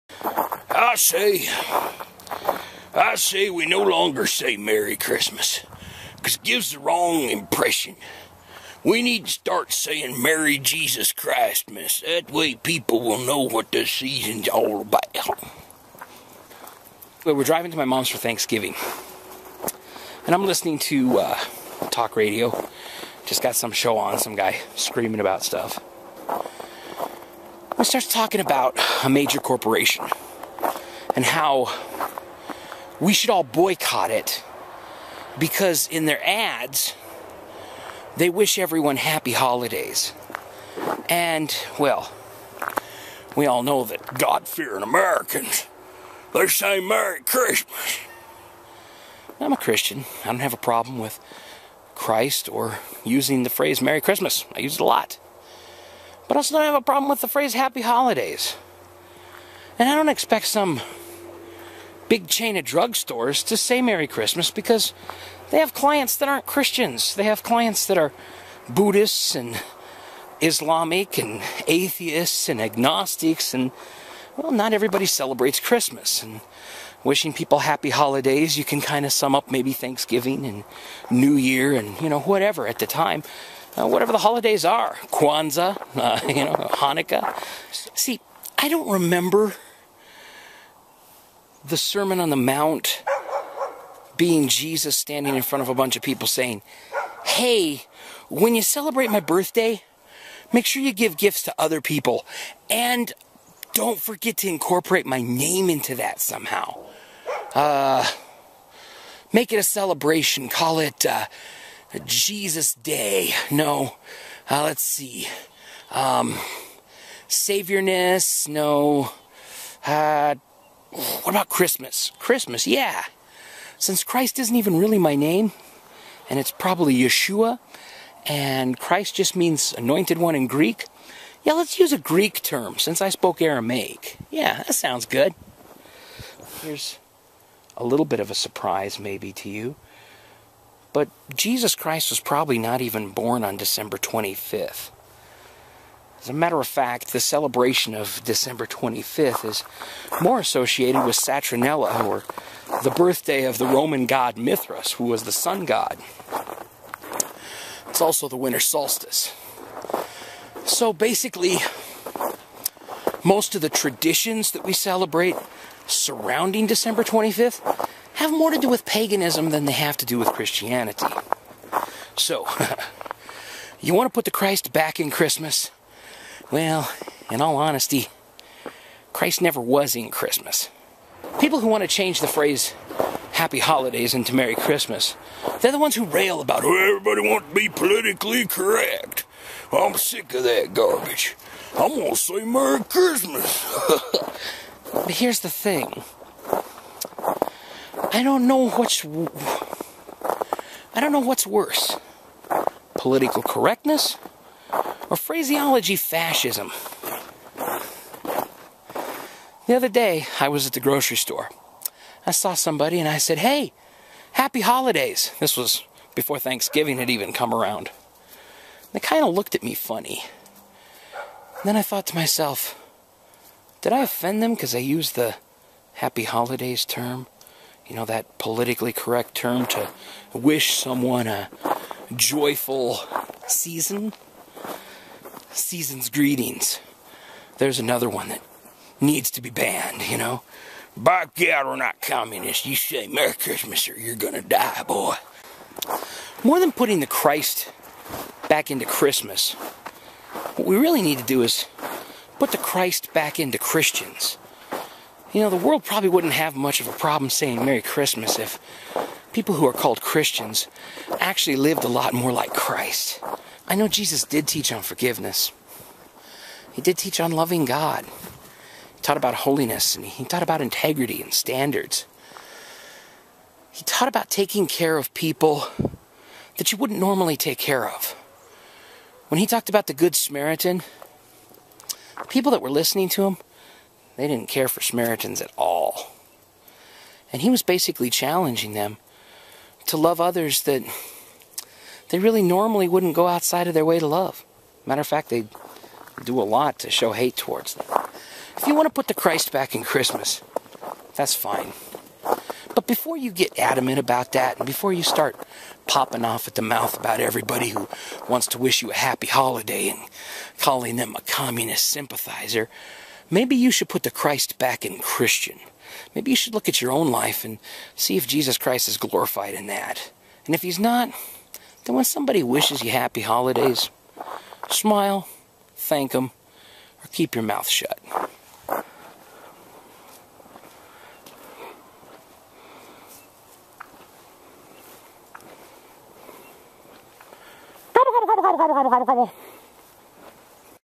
Happy Holidays or Merry Christmas?
My take on the argument that always gets sprung up this time of year.